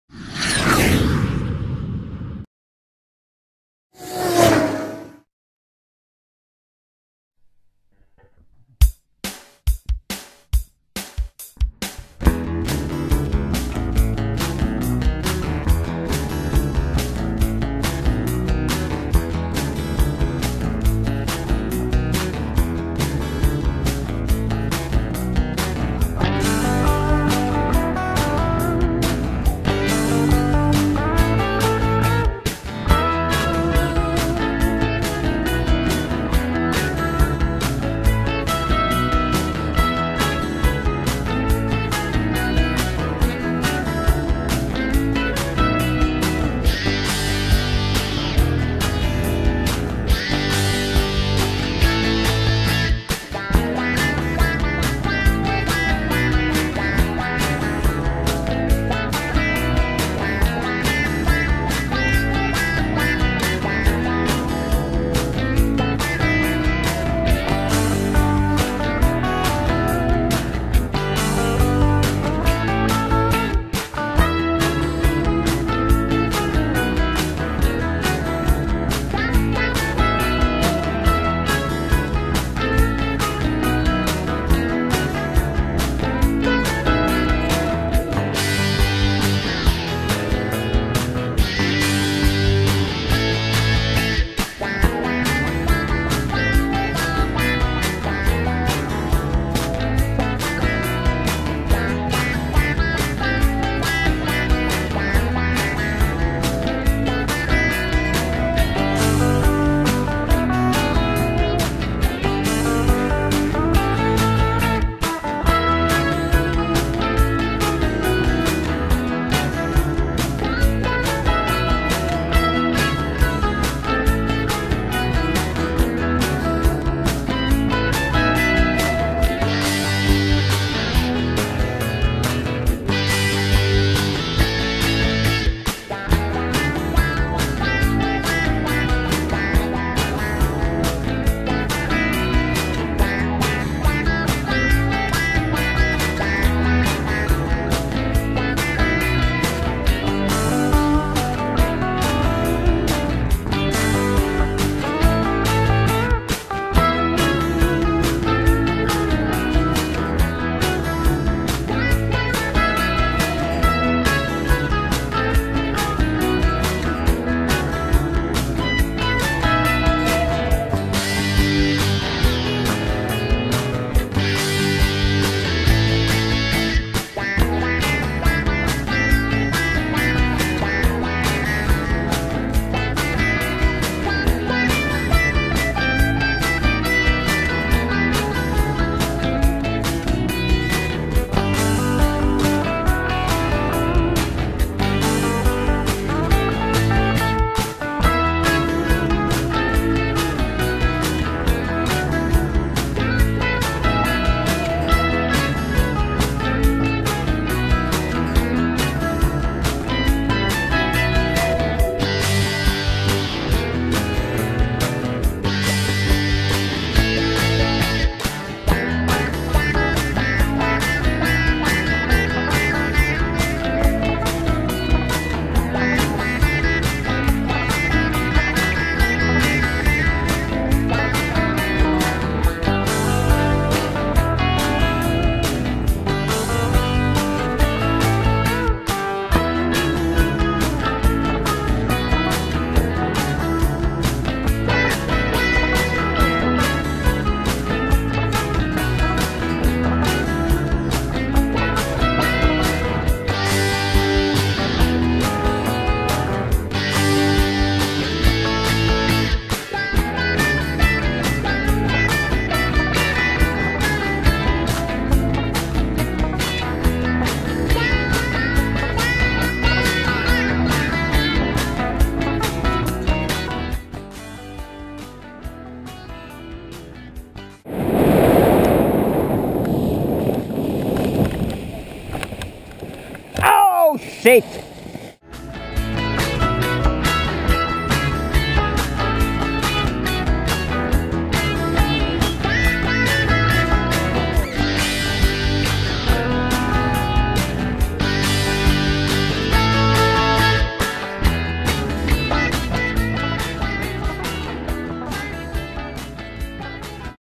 Alicante Paragliding
Paragliding in Alicante Spain december 2013